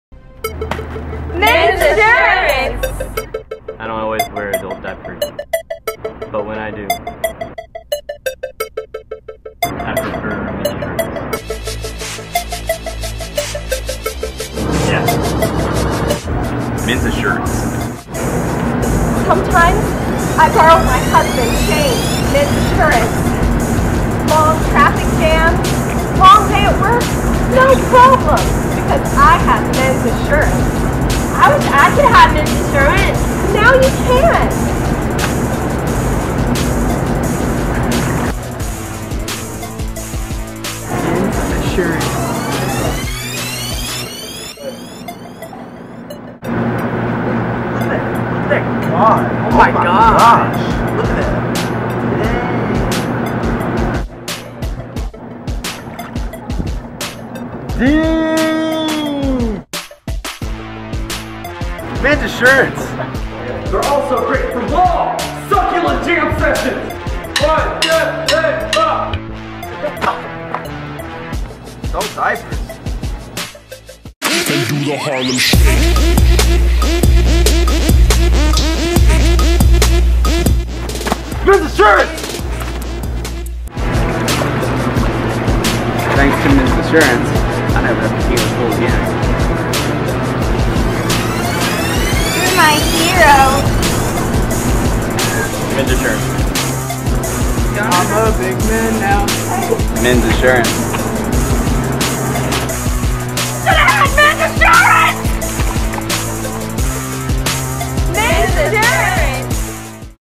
Men's Assurance
Family Group 3 from Echo Beach Camp 2013 makin dat Men's Assurance commercial